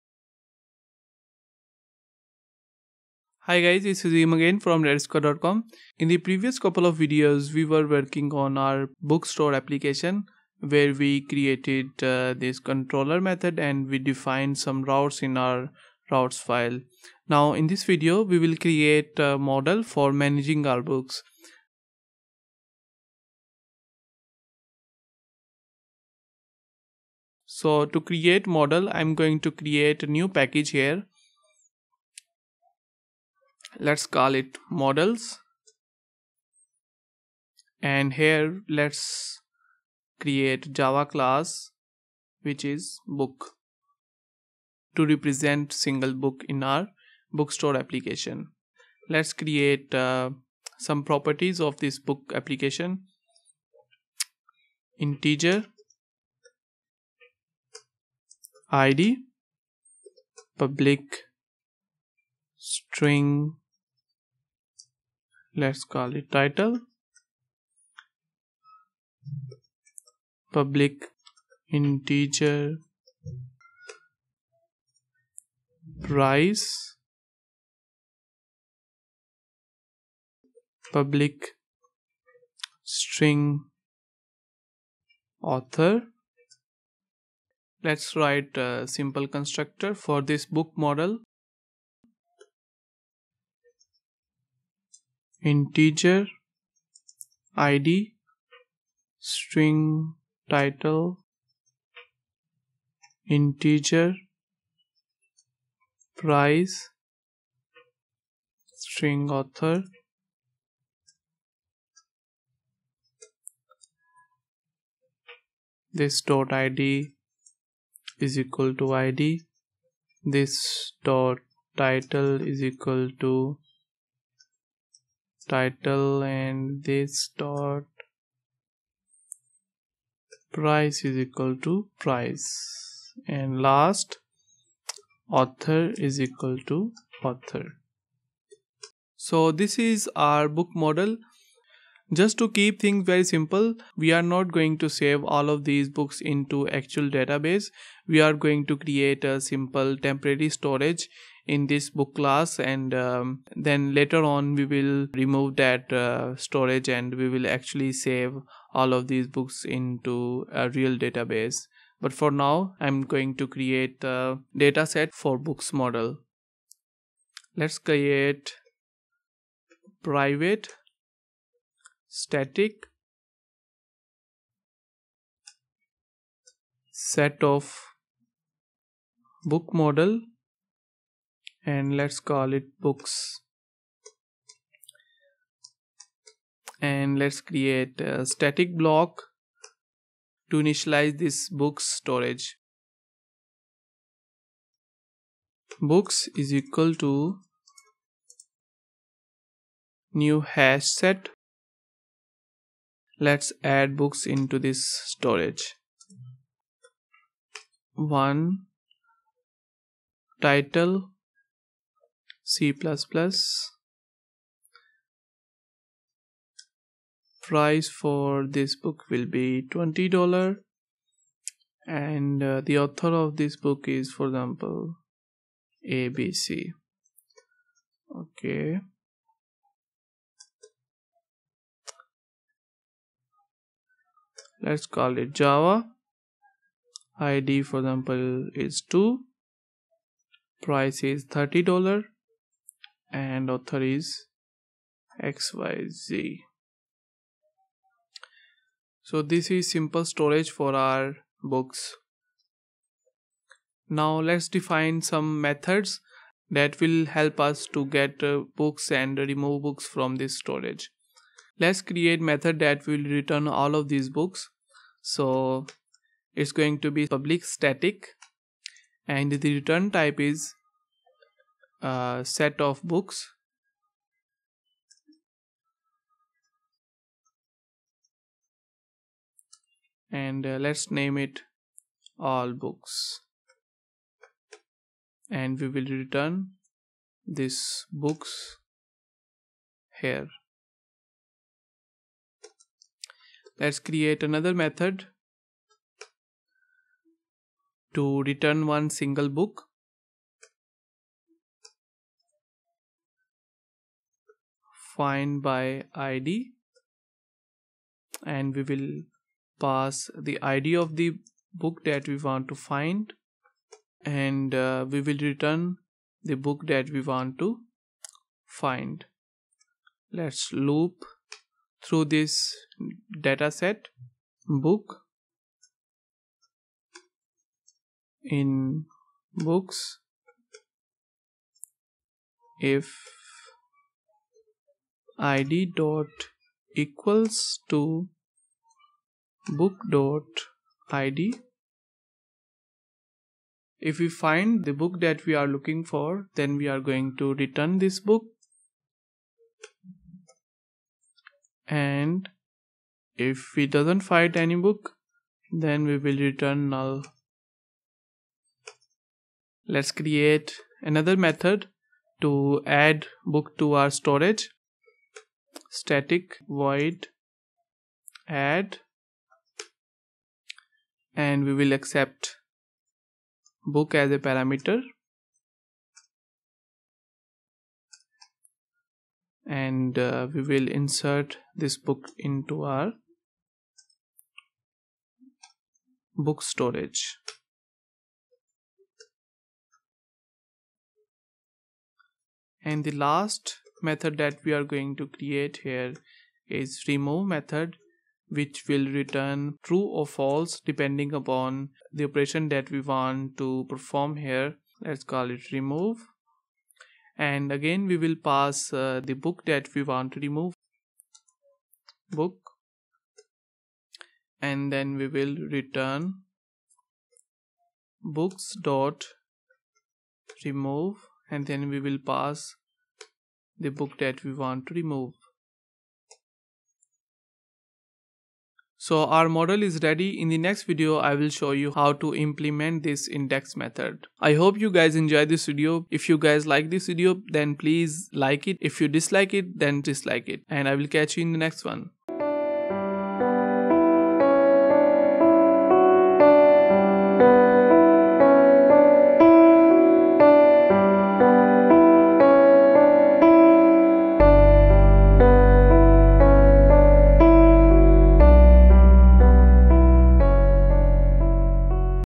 Play Framework Tutorial # 12 | Implement Book Model in Play Framework
In this video, we will implement Model for our Bookstore app and we will also implement following methods inside our Book Model
1-allBooks ( This will return all books )
2-add(To add new Book)
3-remove(To remove a book)

so after this, we will implement methods in our controller and after this, our very first and basic app will be complete :)

You Can Download this App from Github